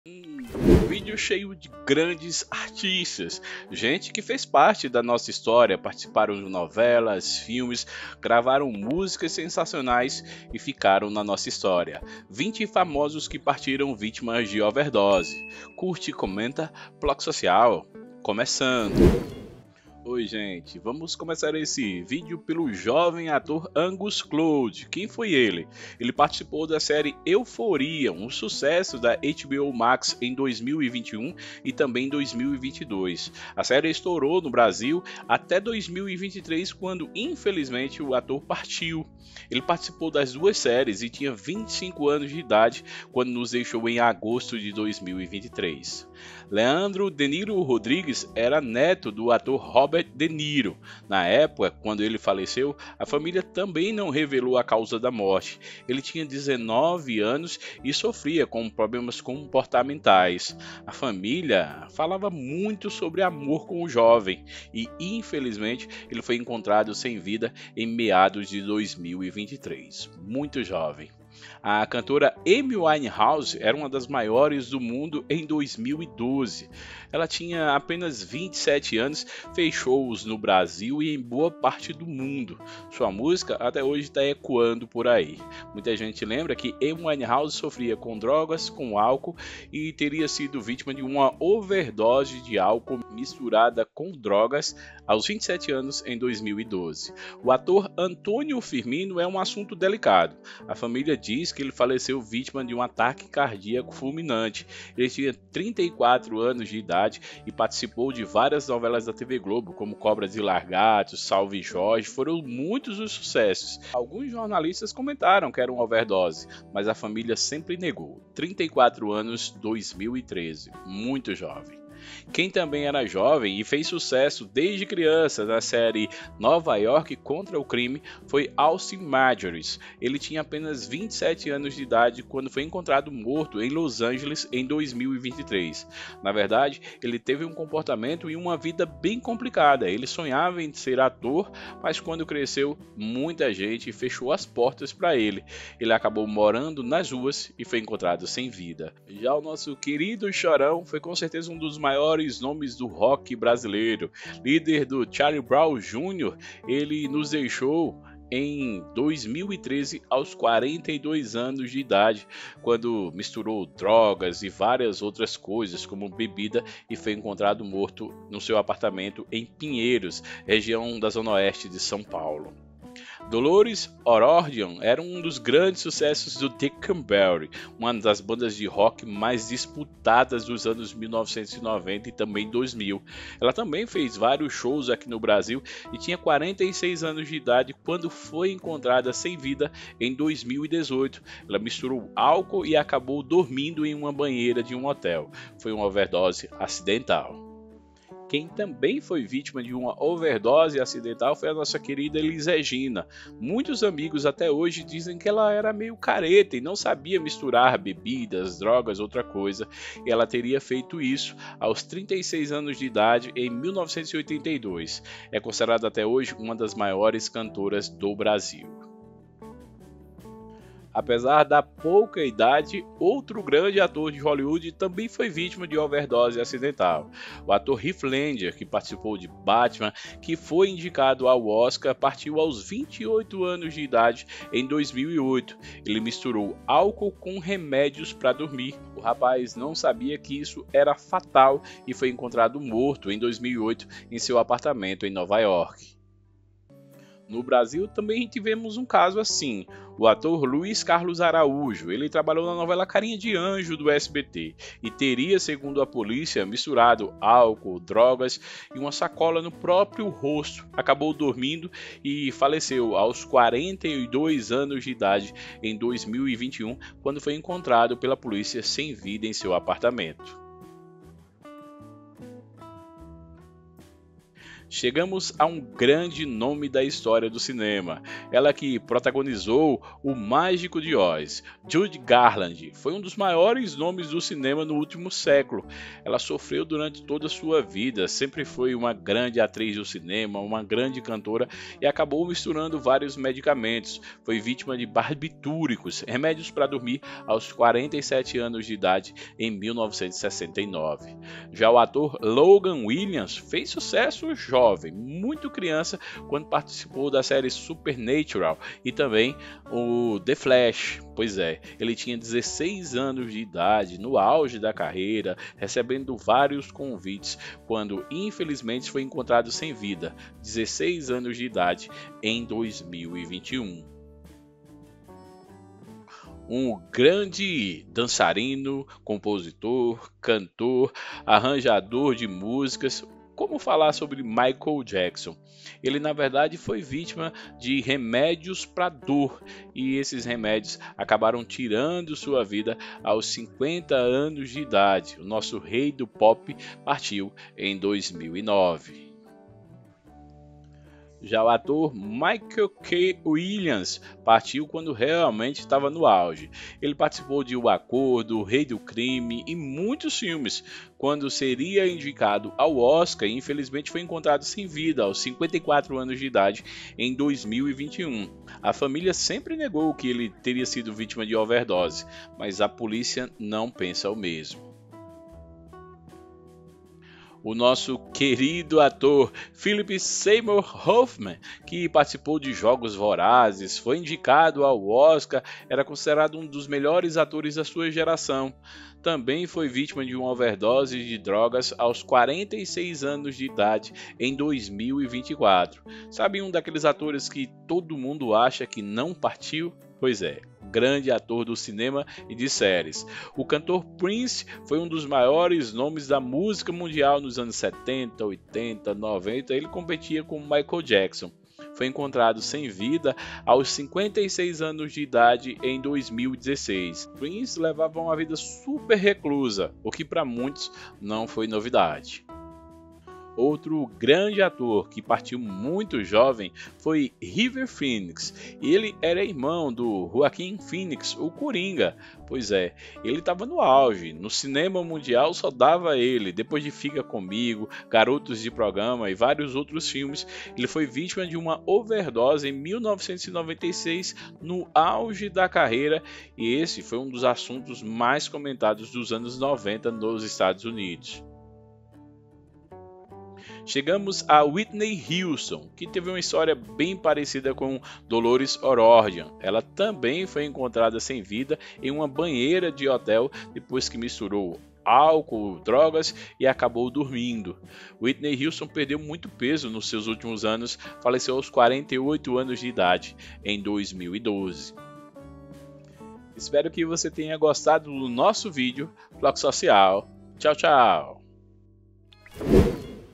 20 famosos que partiram vítimas de 0verd0se e deixaram saudade • Chorão, Michael Jackson, Elis e +
20 famosos que partiram vítimas de 0verd0se e deixaram saudade • Chorão, Michael Jackson, Elis e mais